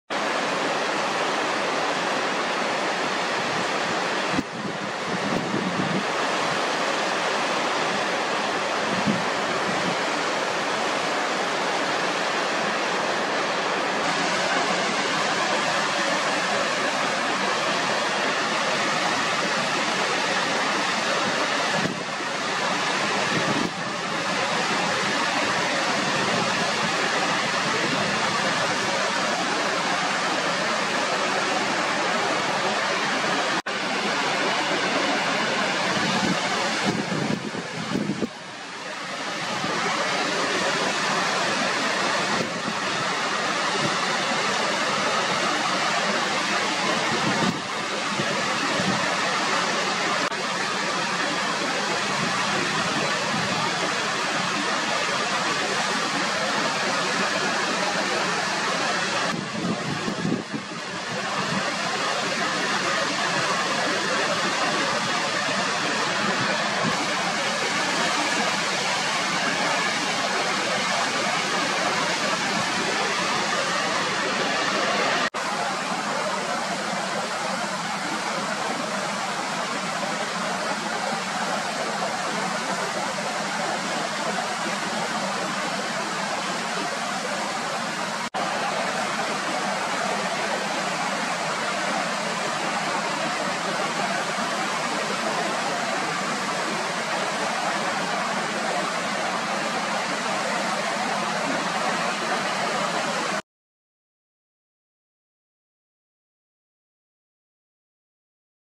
Waterfall cascades off a wide rock face on the climb to Rudragaira pak in Garhwal Himalaya
Somewhere between a waterfall and a stream, and almost like an aesthetic created water feature, an open face waterfall cascades off a wide rock face on the climb to Rudragaira peak in the Garhwal Himalaya of Uttarakhand. The waters flow into the Ganges downstream from the Rudragaira torrent.